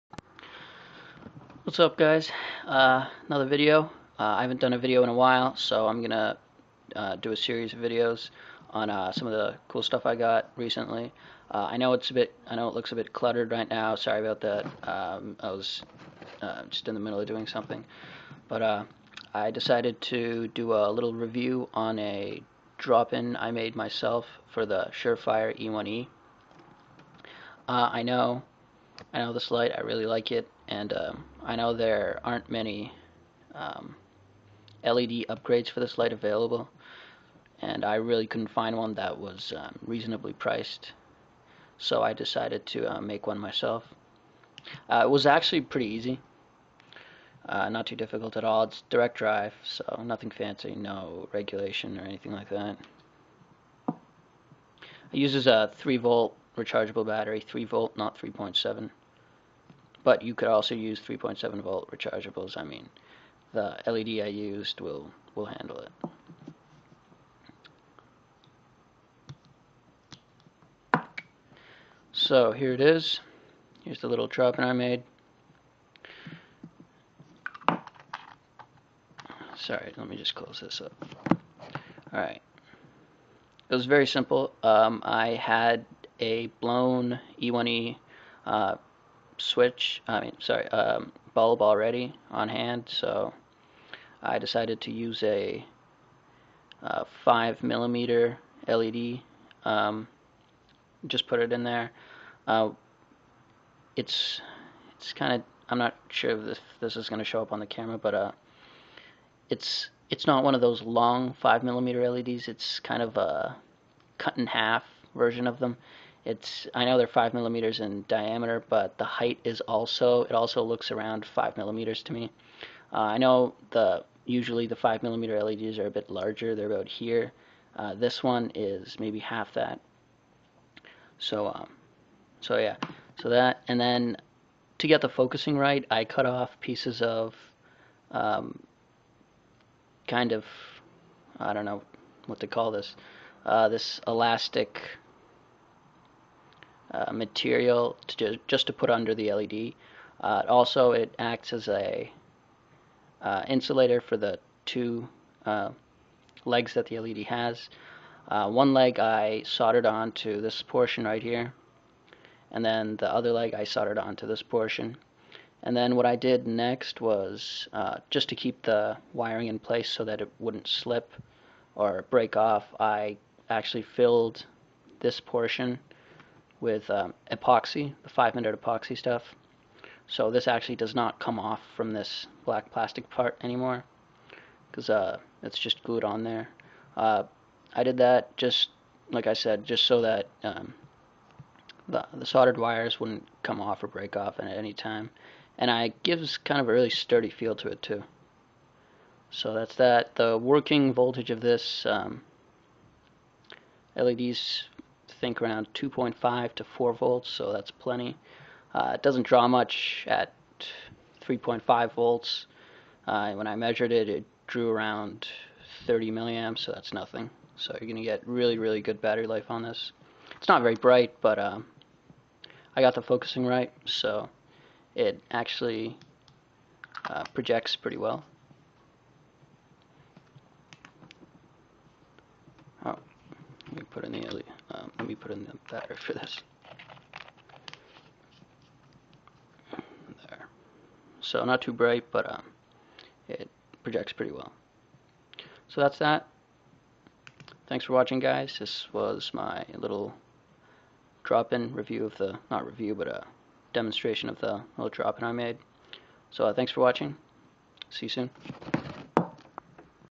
Simple SUREFIRE E1E LED drop in...
A simple direct drive LED drop-in I made for the Surefire e1e. Draws 20mA @ 3.4V. I'd say its around 8-15 lumens. RATE ANS SUB!!